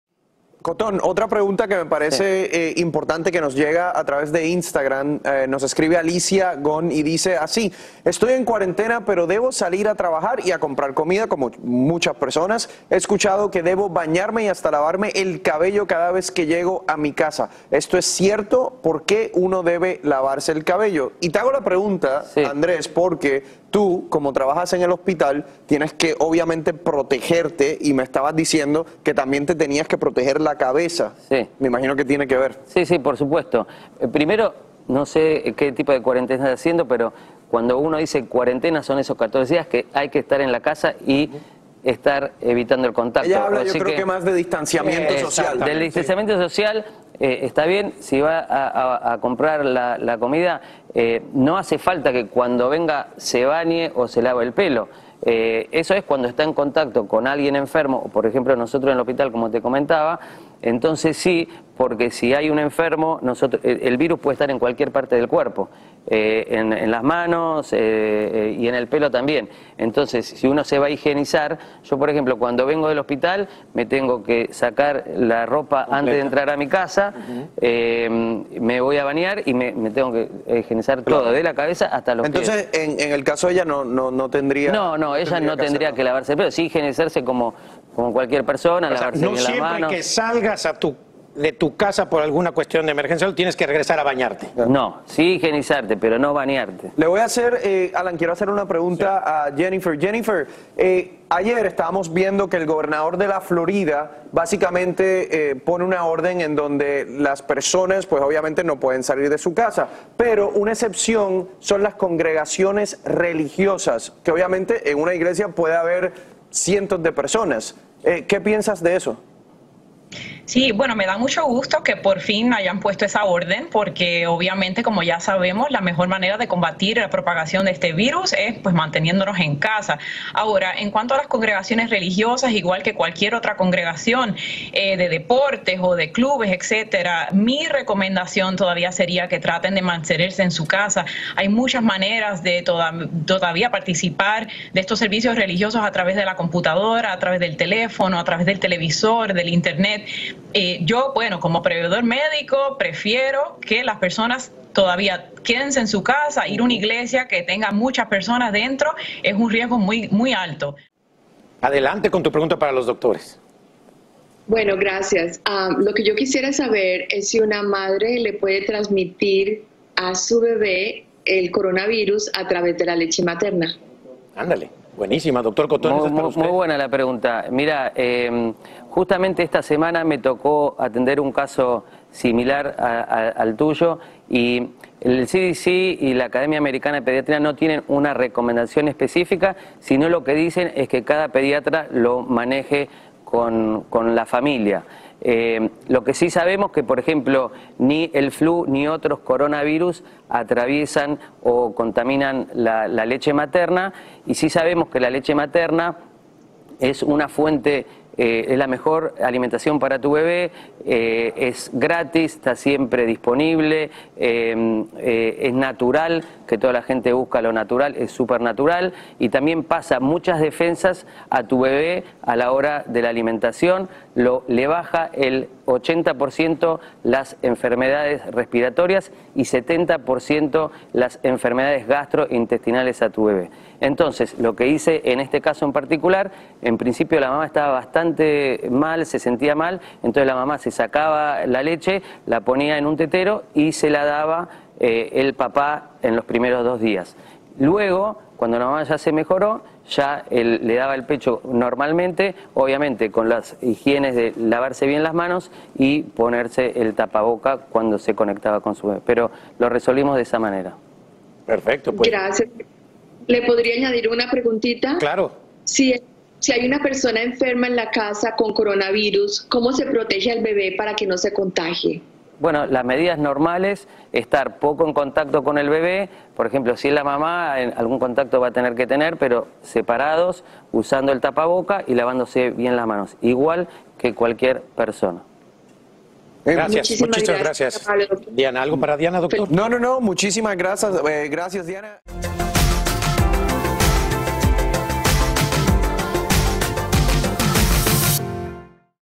Si salgo durante la cuarentena, ¿debo lavarme el cabello al volver? | Dr. Juan
Una de las dudas más comunes a la que se enfrentan nuestros médicos expertos es si una persona debe lavarse el cabello después de haber salido a la calle en estos tiempos de cuarentena por coronavirus. ¿Realmente es necesario? Esta es la opinión del Dr. Andrés Cotton.